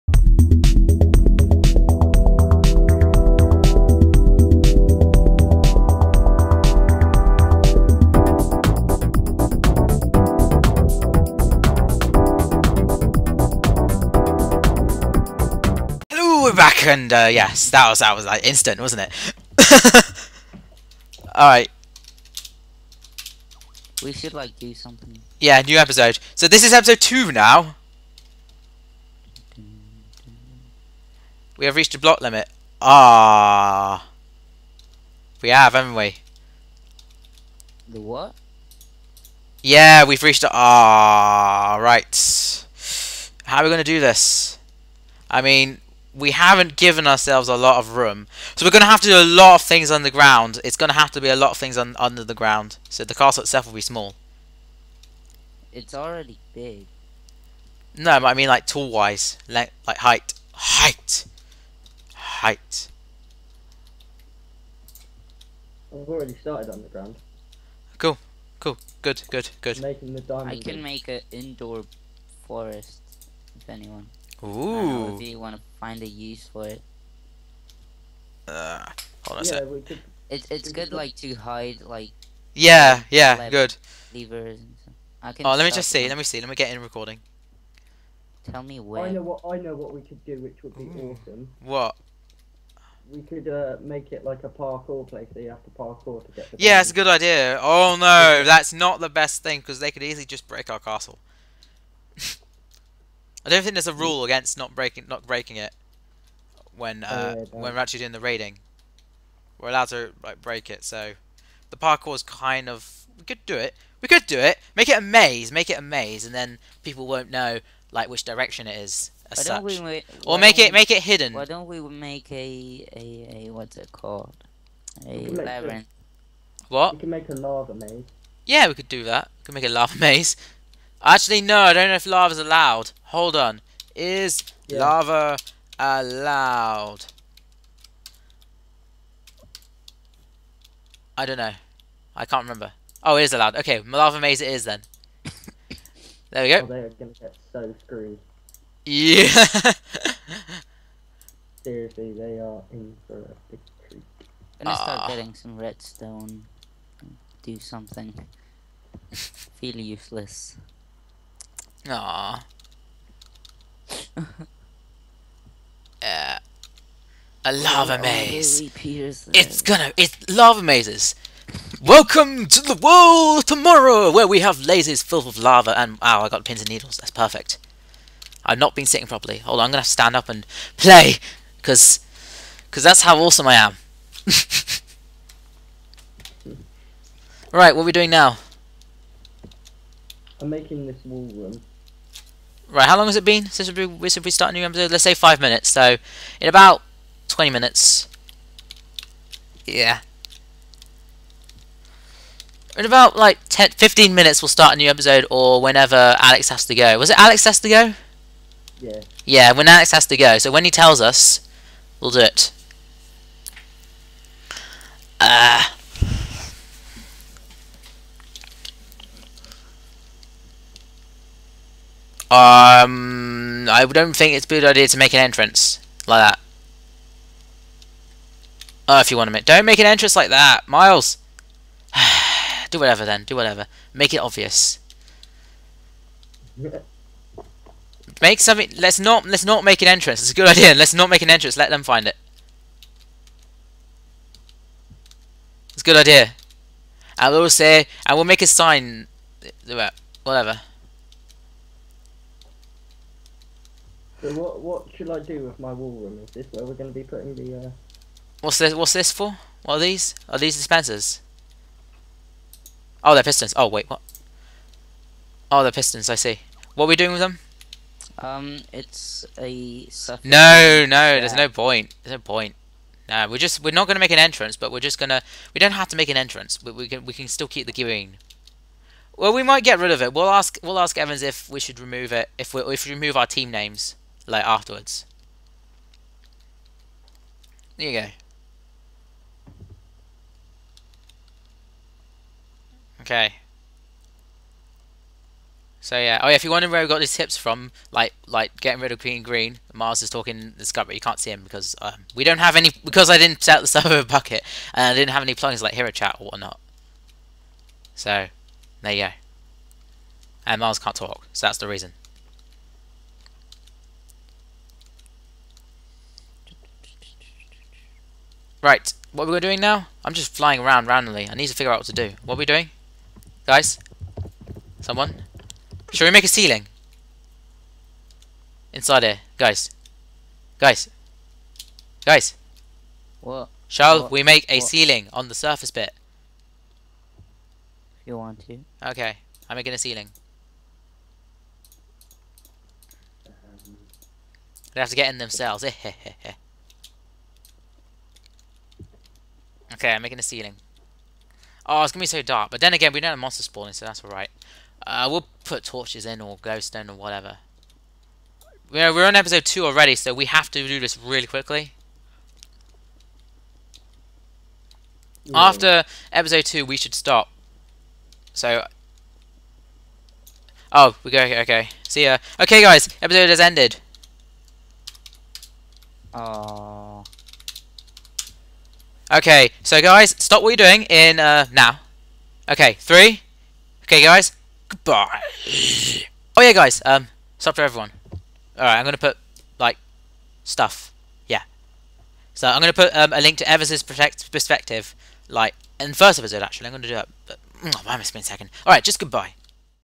Capture The Diamond - Season 1: Episode 2 (Green Team)
Episode 2: A Short Spark of Progress...

Welcome to the second episode of our highly anticipated series. I'm glad you could join us. ;) In this series, we split into two teams (Green and Blue), build a castle for 5 episodes, hide a diamond in our respective castles, and raid the other team's castle for the next 5 episodes. The first team to get the diamond wins. Simple. ;D

In this episode, we build a ceiling, pistons, and something colourful. Thanks, AkashV22.